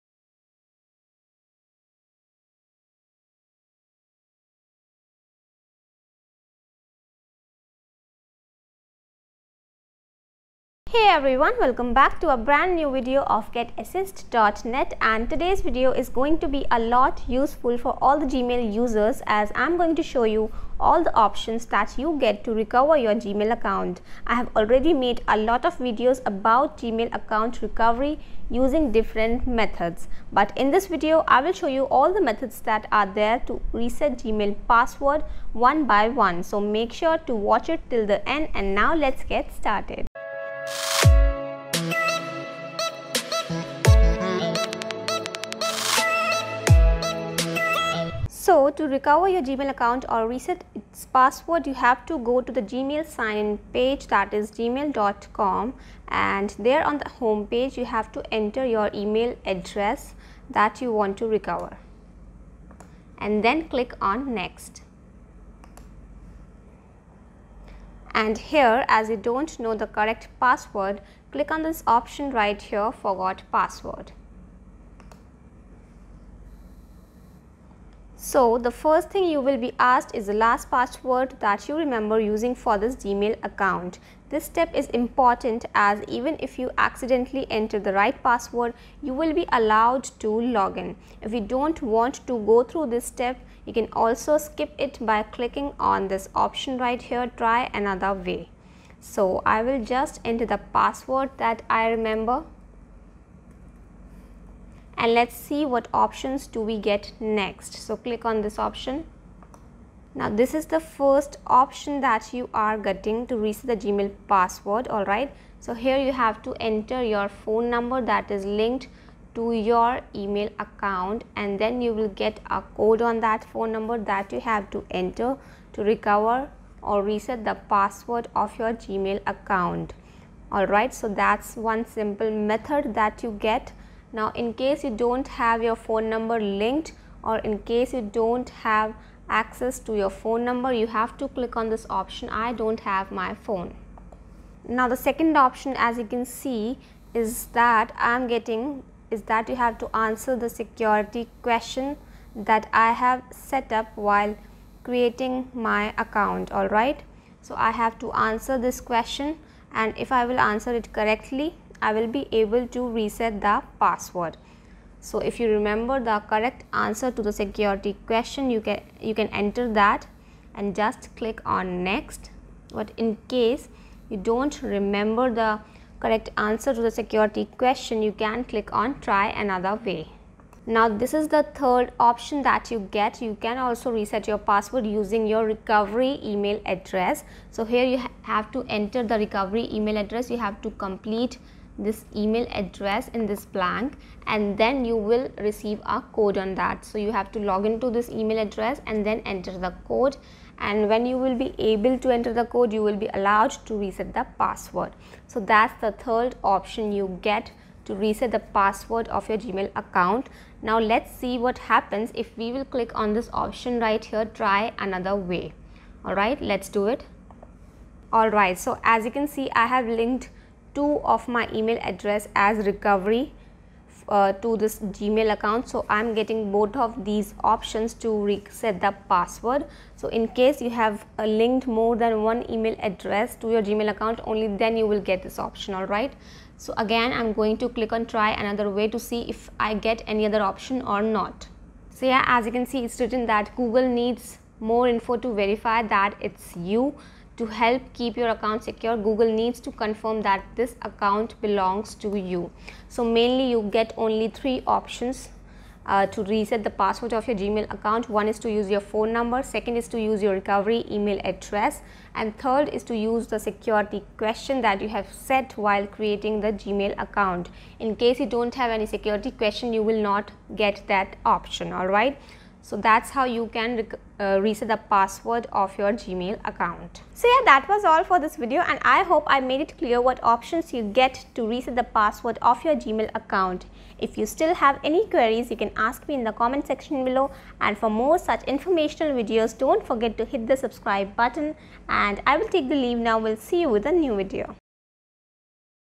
All Possible Ways to Recover a Gmail Account
If you have lost your Gmail account and you are not able to recover your Gmail account then you are in the right place.

Today we will show you all the possible ways by which you can recover your Gmail account, although we have already created many videos on Gmail recovery and we will also provide you the link for our Gmail playlist.